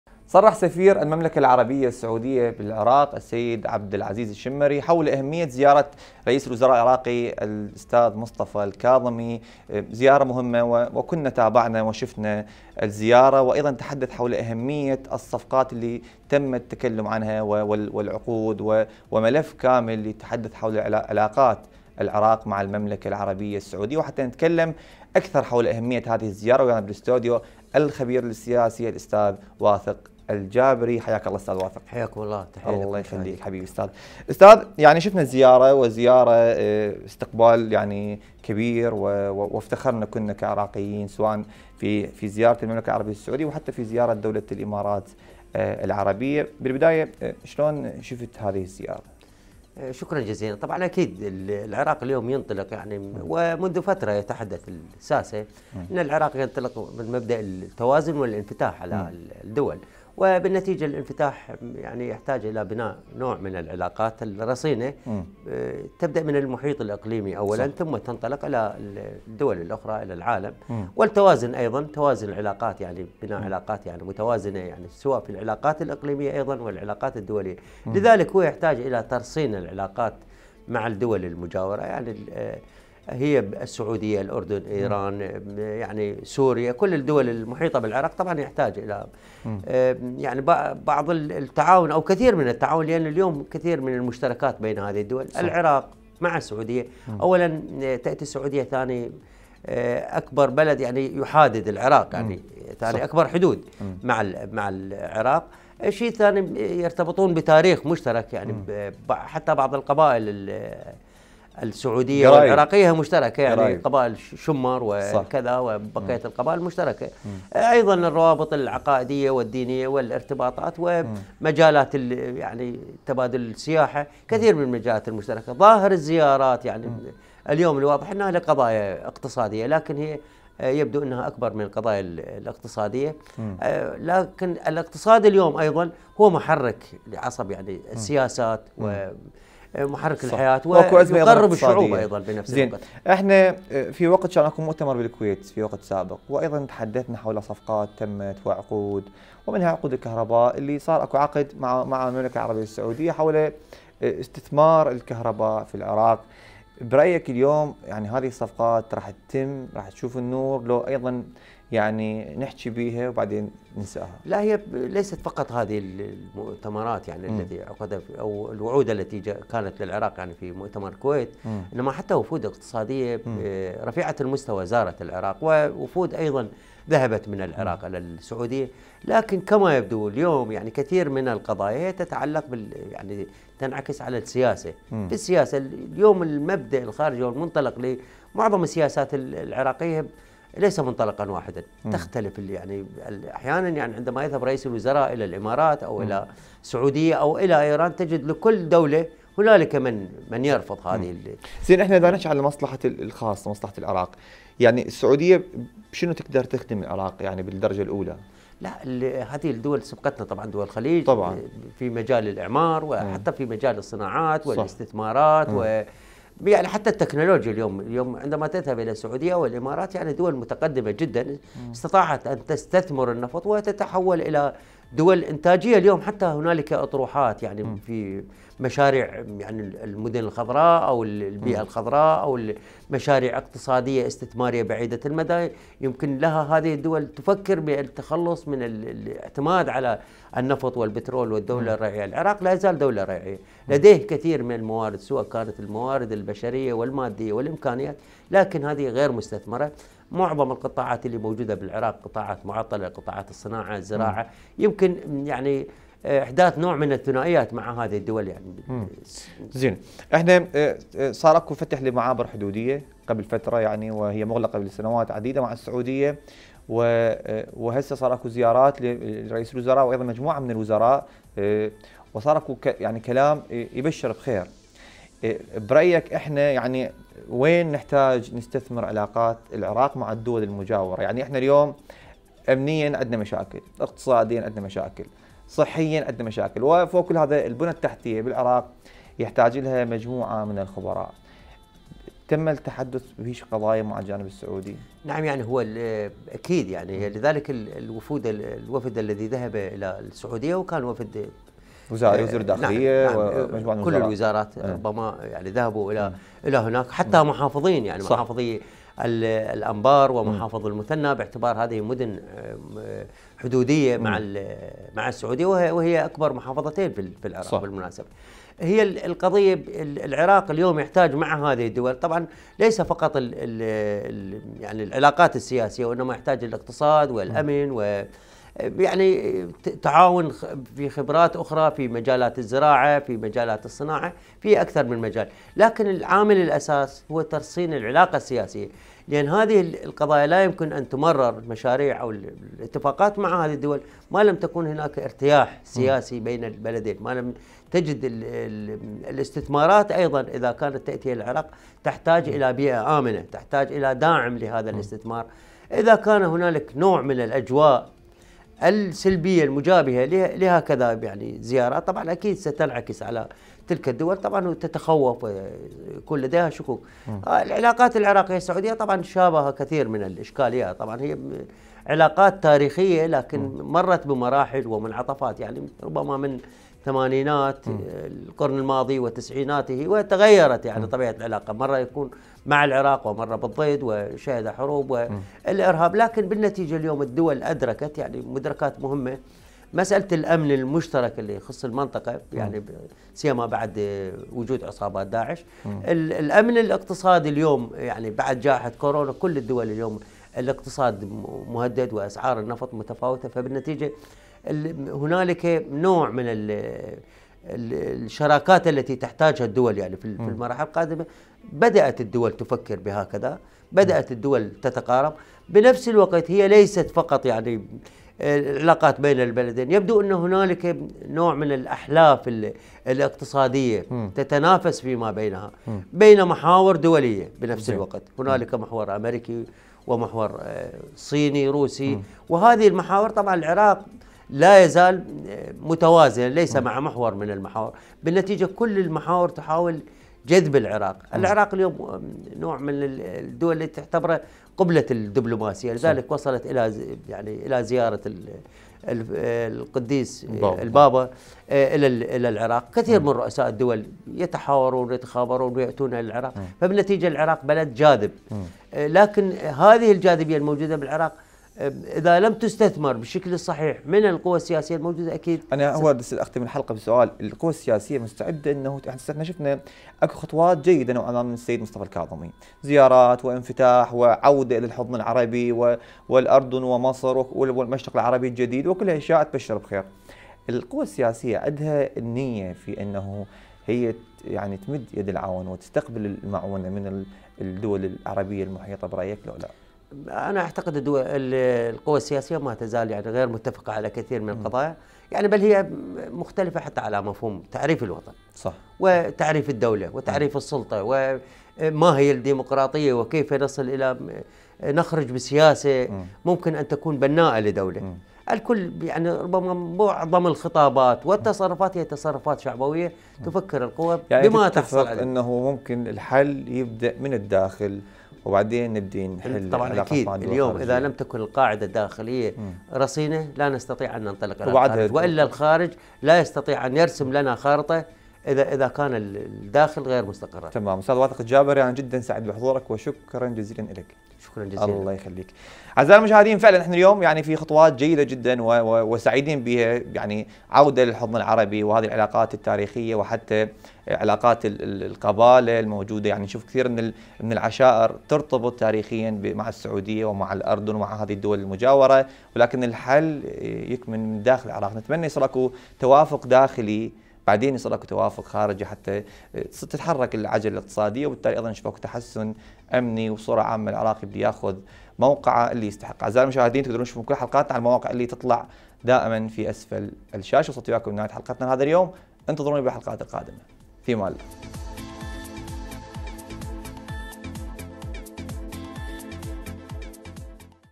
صرح سفير المملكة العربية السعودية حول اهمية زيارة الكاظمي الى المملكة
مع الخبير السياسي جابر الواثق وحديث عن اهمية زيارة الكاظمي الى المملكة العربية السعودية